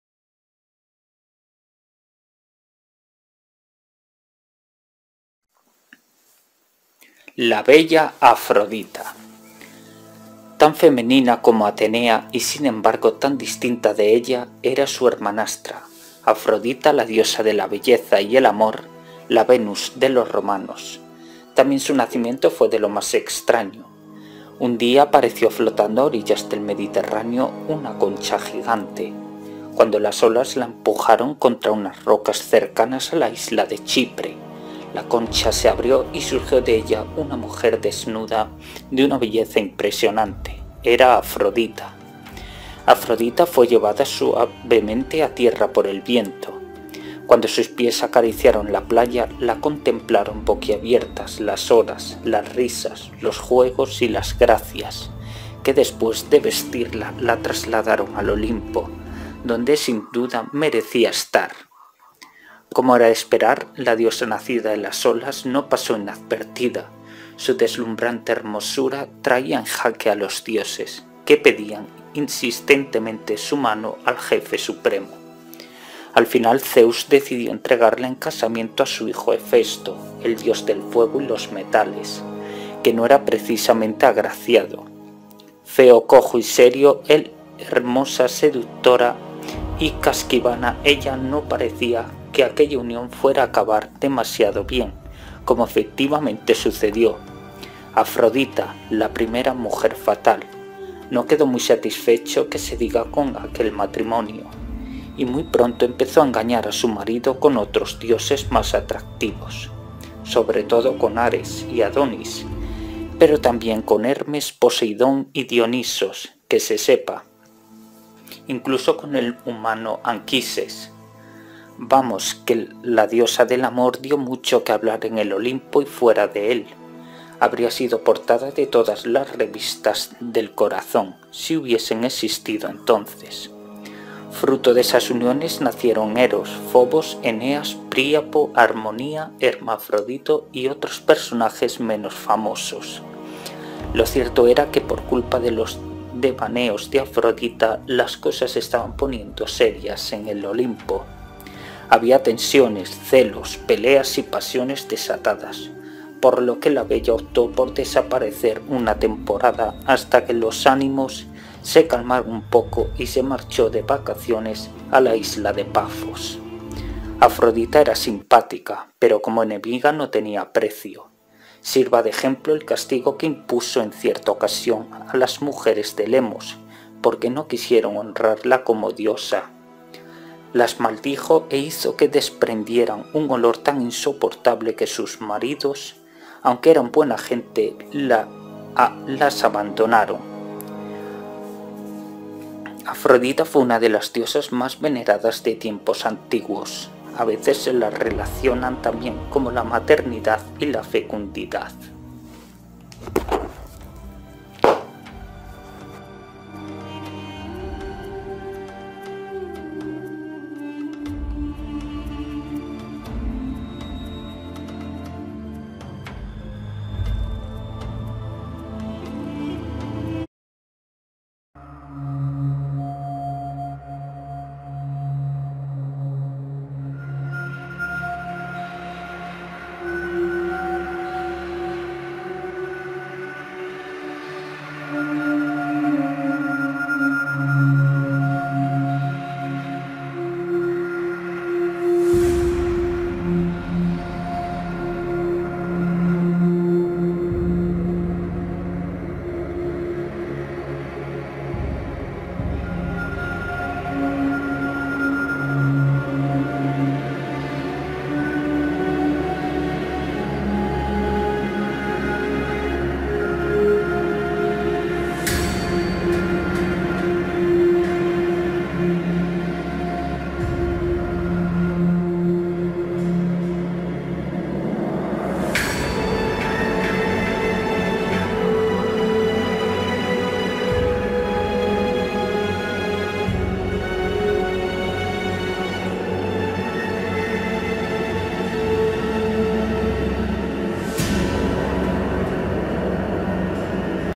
afrodita diosa del amor mitologia griega
mito de afrodita mitologia griega. afrodita es la diosa de la belleza y el amor es tambien la reproduccion mas practica y ella nacio de una concha en el mar despues de la castracion de urano.